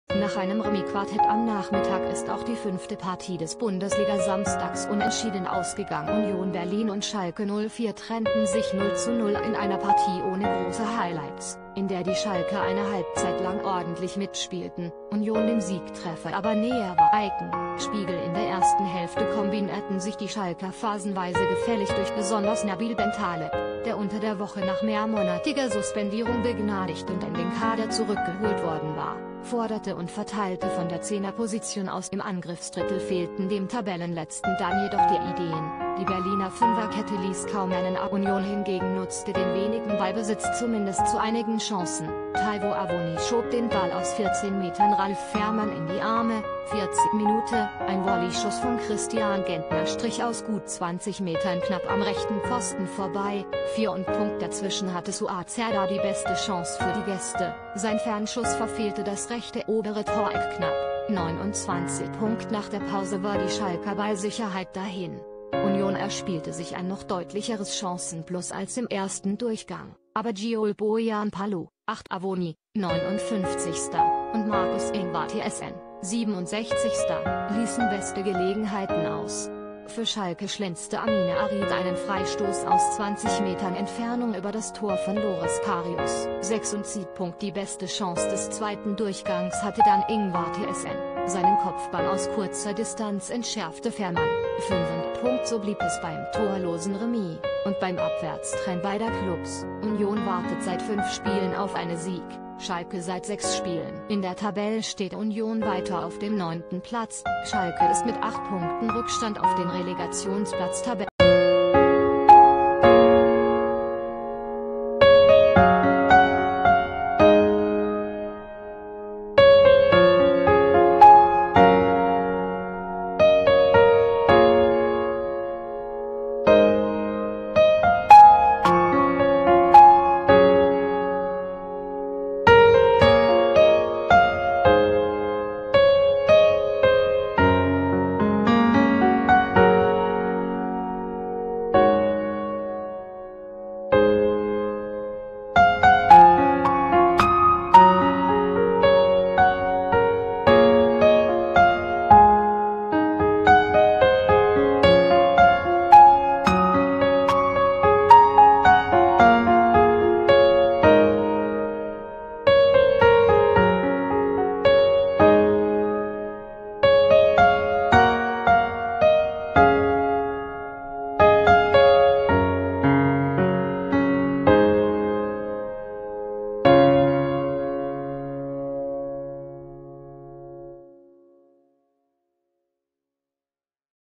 Fußball-Bundesliga: Union Berlin und Schalke 04 treten auf der Stelle
Nach einem Remis-Quartett am Nachmittag ist auch die fünfte Partie des Bundesliga-Samstags unentschieden ausgegangen. Union Berlin und Schalke 04 trennten sich 0:0 in einer Partie ohne große Highlights, in der die Schalker eine Halbzeit lang ordentlich mitspielten, Union dem Siegtreffer aber näher war.  Icon: Spiegel   In der ersten Hälfte kombinerten sich die Schalker phasenweise gefällig durchs Mittelfeld. Besonders Nabil Bentaleb, der unter der Woche nach mehrmonatiger Suspendierung begnadigt und in den Kader zurückgeholt worden war, forderte und verteilte von der Zehnerposition aus den Ball. Im Angriffsdrittel fehlten dem Tabellenletzten dann jedoch die Ideen, die Berliner Fünferkette ließ kaum einen Abschluss zu. Union hingegen nutzte den wenigen Ballbesitz zumindest zu einigen Chancen: Taiwo Awoniyi schob den Ball aus 14 Metern Ralf Fährmann in die Arme (14. Minute), ein Volleyschuss von Christian Gentner strich aus gut 20 Metern knapp am rechten Pfosten vorbei (34.). Dazwischen hatte Suat Serdar die beste Chance für die Gäste, sein Fernschuss verfehlte das rechte obere Toreck knapp (29.).   Nach der Pause war die Schalker Ballsicherheit dahin. Union erspielte sich ein noch deutlicheres Chancenplus als im ersten Durchgang, aber Joel Pohjanpalo (58.), Awoniyi (59.) und Marcus Ingvartsen (67.) ließen beste Gelegenheiten aus. Für Schalke schlenzte Amine Harit einen Freistoß aus 20 Metern Entfernung über das Tor von Loris Karius (76.). Die beste Chance des zweiten Durchgangs hatte dann Ingvartsen, seinen Kopfball aus kurzer Distanz entschärfte Fährmann (85.). So blieb es beim torlosen Remis – und beim Abwärtstrend beider Klubs: Union wartet seit fünf Spielen auf eine Sieg, Schalke seit sechs Spielen. In der Tabelle steht Union weiter auf dem neunten Platz, Schalke ist mit acht Punkten Rückstand auf den Relegationsplatz Tabellenletzter.